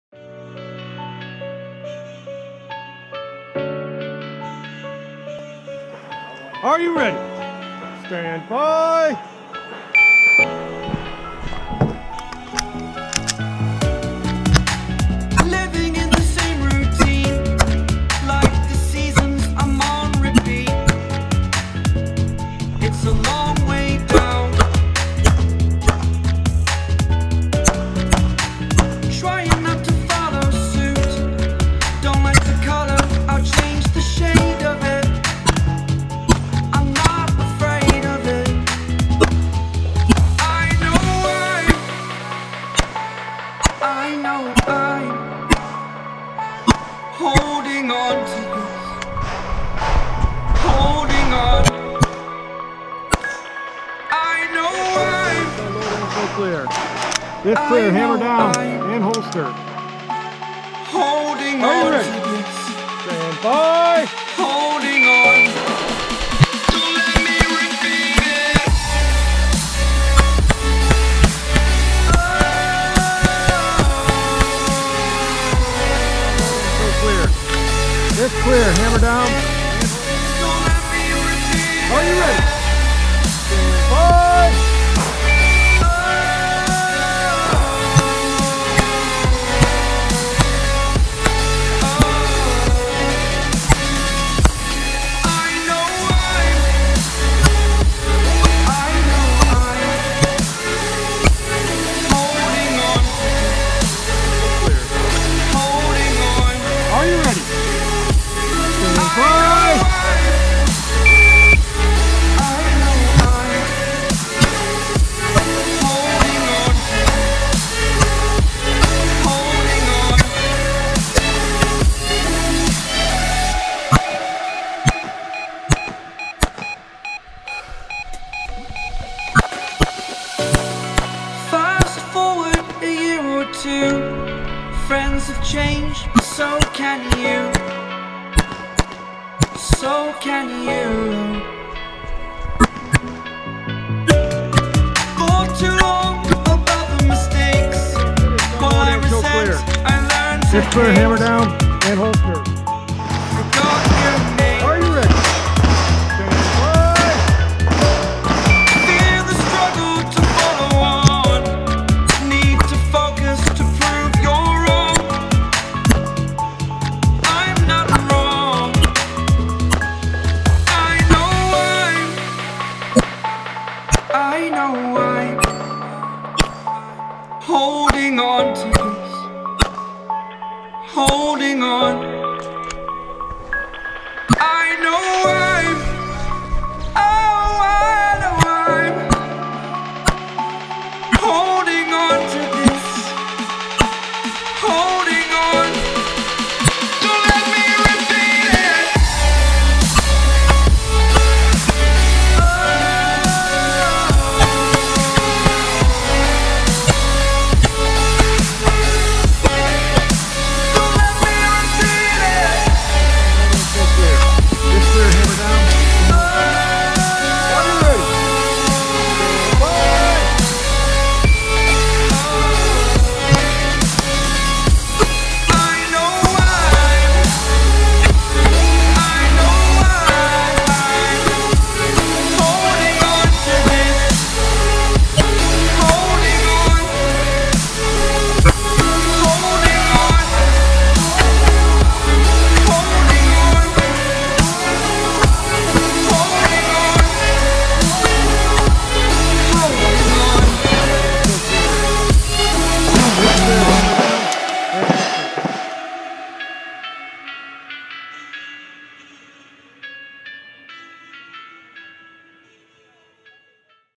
MRPC USPSA 7 22 2018
More suck with the Carry Optics gun...just can't quite get the hang of that thing. Still more fun than going to work, though.